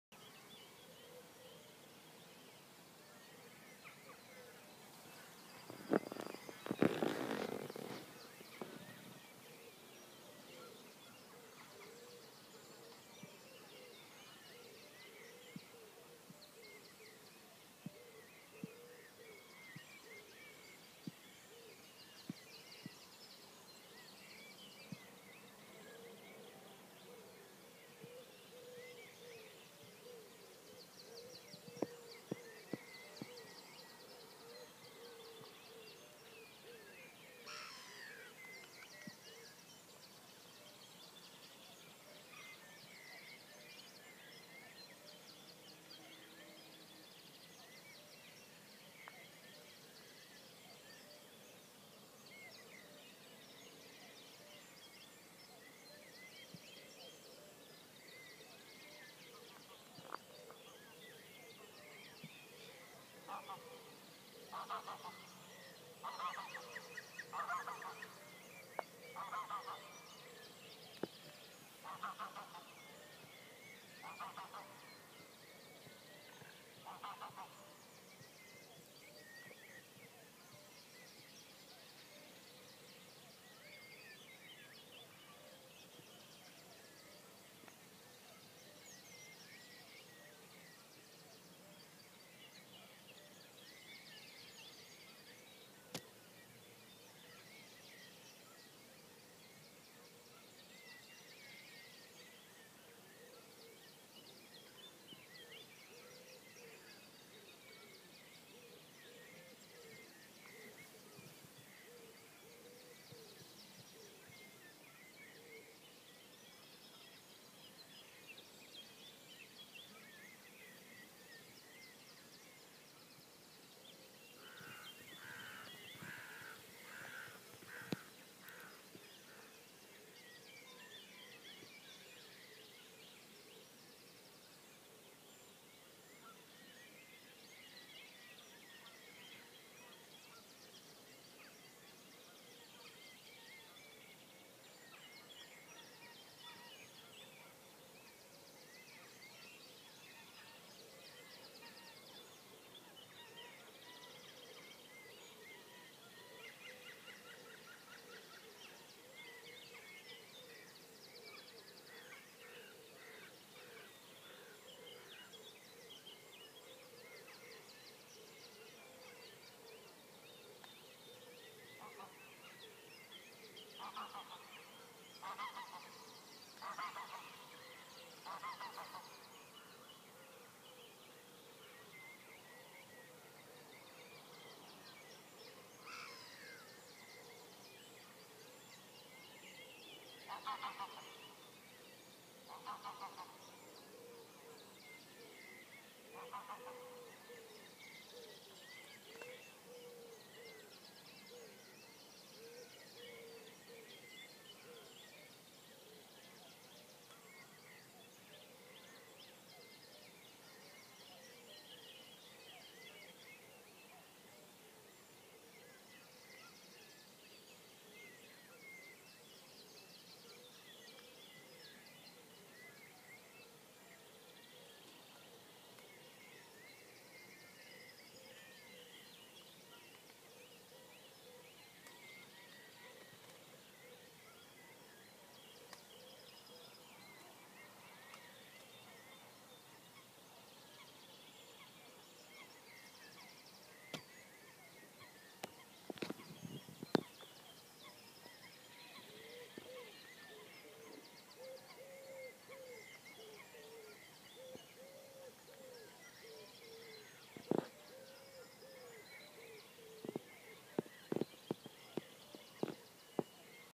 St Johns lake Richworth linear
Dawn breaking over st johns lake. Richworth linear nr. Oxford